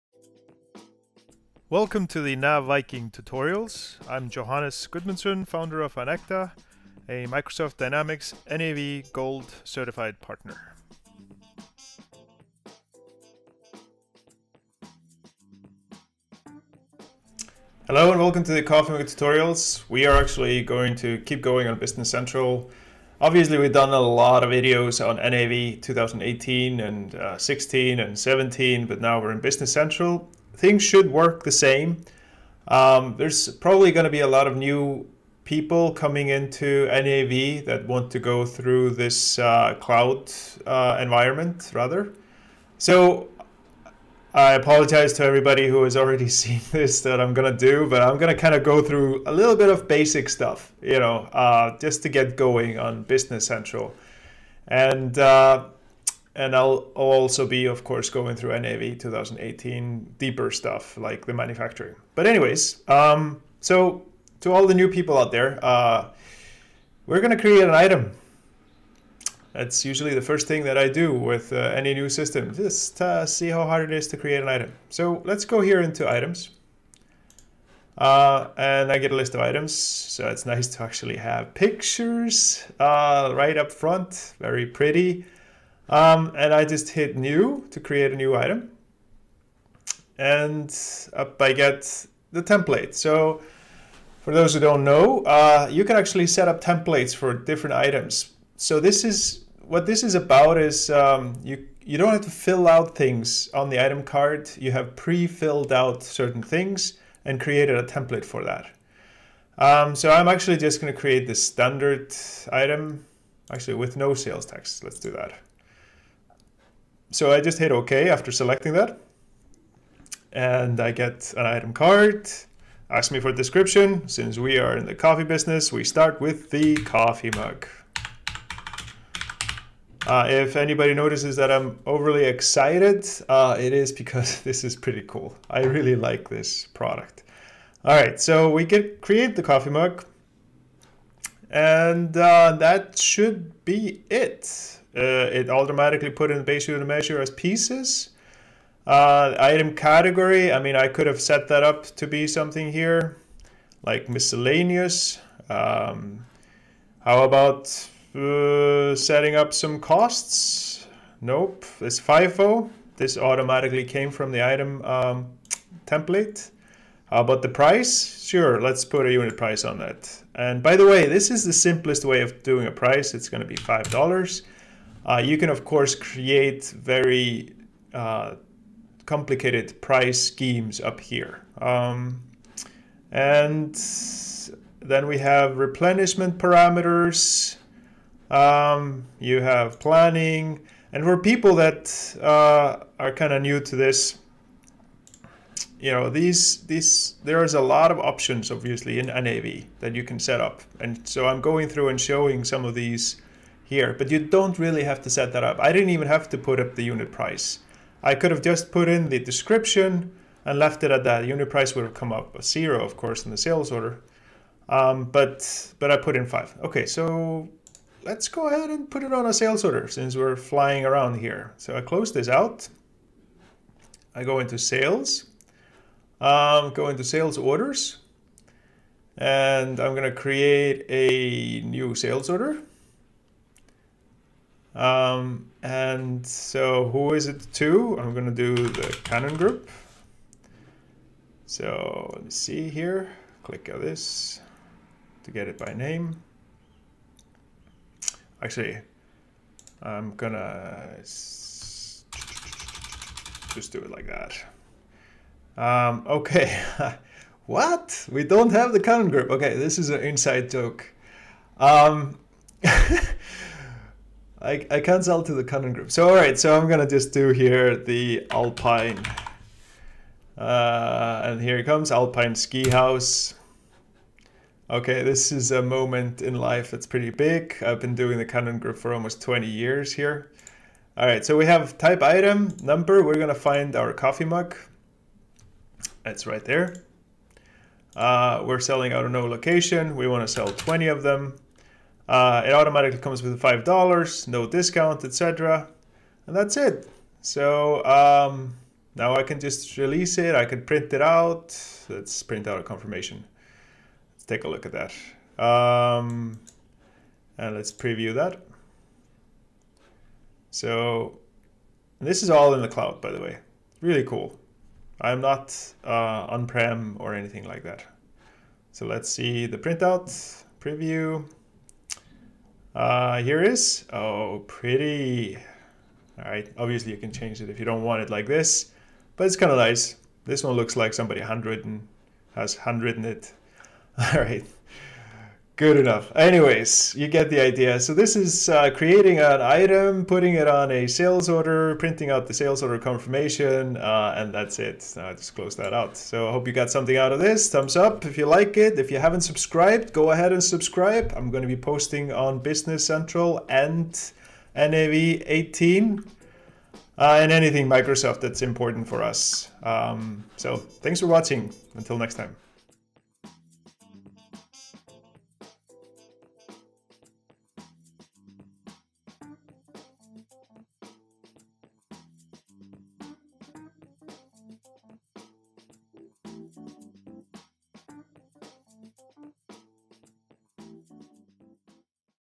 Business Central: Create an Item
Learn how to create an item and sales order in Microsoft Dynamics 365 Business Central. With Johannes Gudmundsson in this coffee mug tutorial.